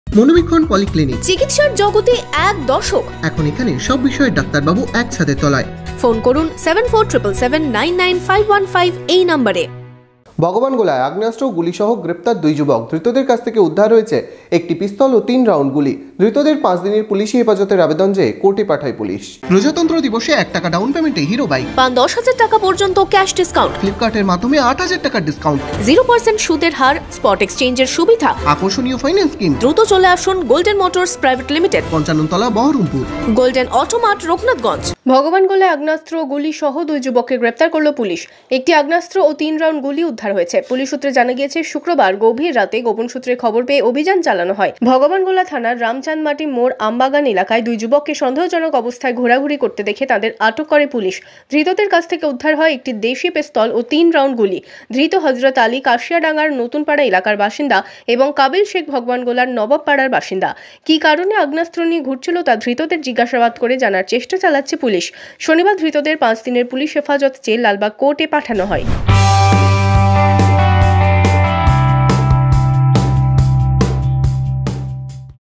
Bhagawangola News: ভগবানগোলায় আগ্নেয়াস্ত্র ও গুলি সহ গ্রেপ্তার দুই যুবক
ভগবানগোলায় আগ্নেয়াস্ত্র ও গুলি সহ গ্রেপ্তার দুই যুবক

Madhyabanga News